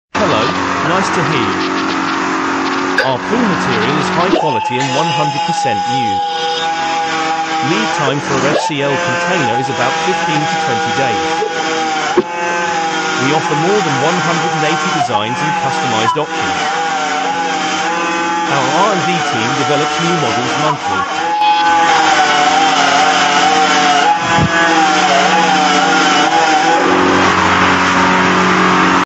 Artificial polyurethane stone panel faux wall PU cultural stone for KTV decor from China
Material: High-quality PU,100% new material, didn’t use recycle and crashed material. Sufficient raw material store, fast delivery

Sizes : 1200x600mm(Running size), 600x300mm or customized

Size Tolerance: Length & Width ± 0.3mm (Strict QA standard)

Styles: More than 180 designs and support customized designs